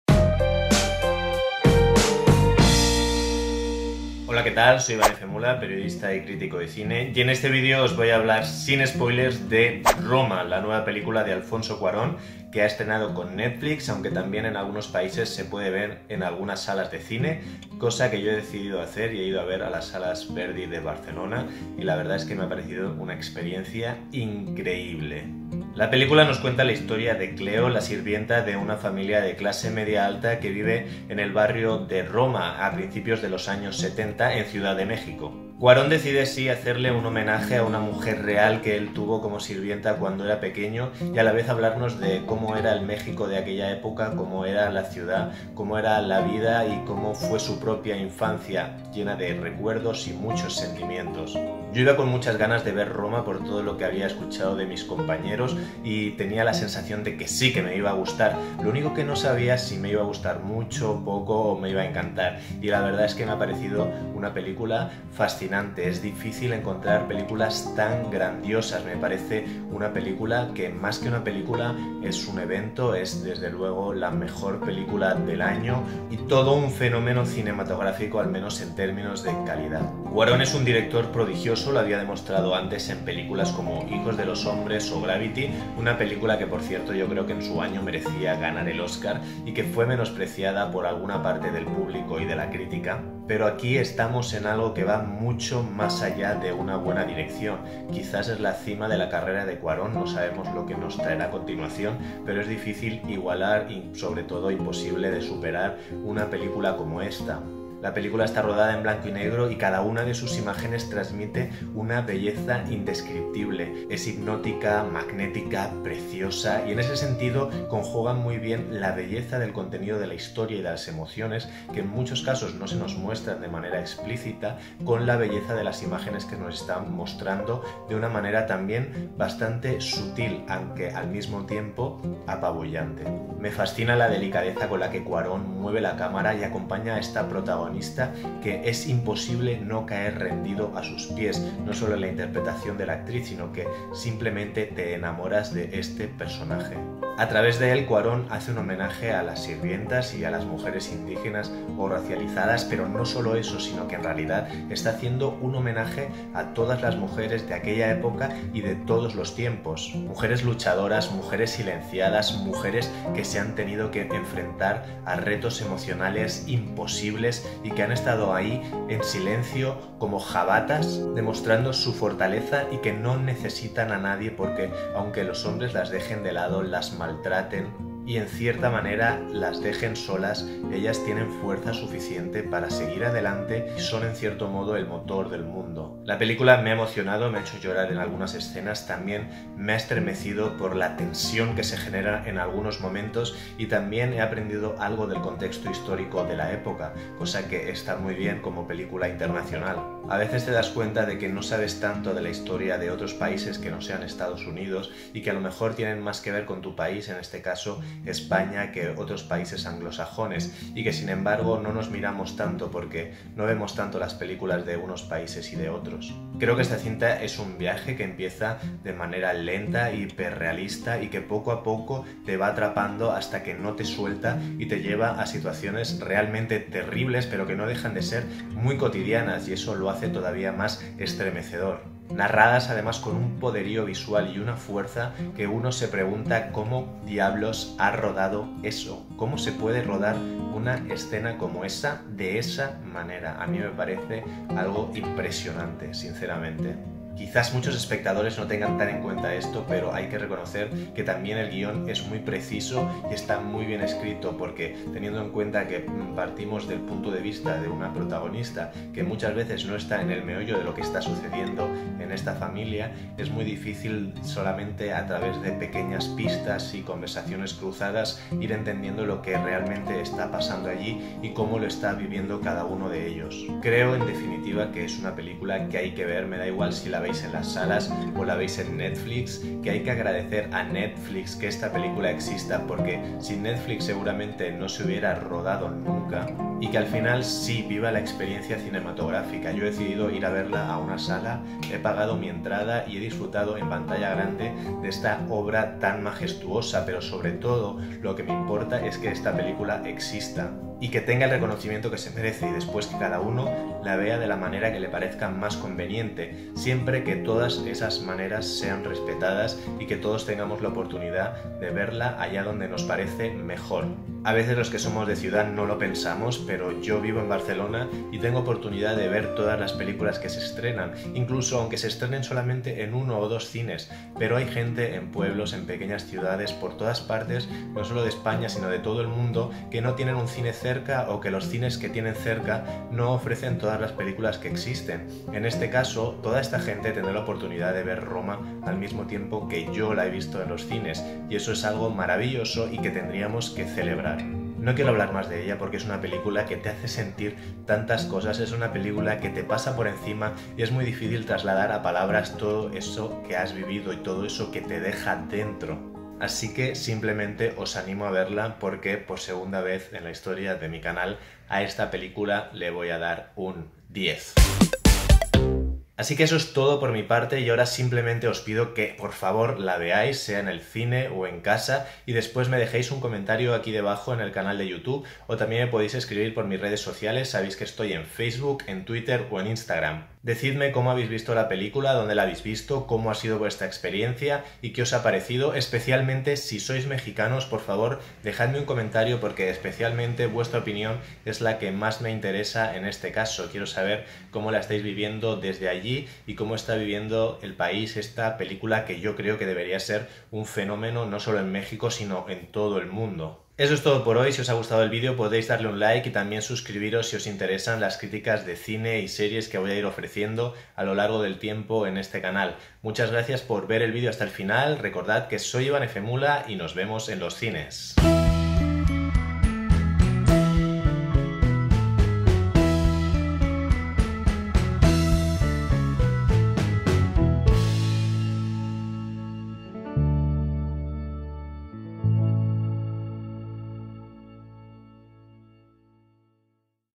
Roma | Netflix | Crítica / Review
Crítica, review, reseña u opinión de "Roma", la película de Netflix protagonizada por Yalitza Aparicio,  Marina de Tavira,  Marco Graf,  Diego Cortina Autrey,  Carlos Peralta,. Dirigida por Alfonso Cuarón.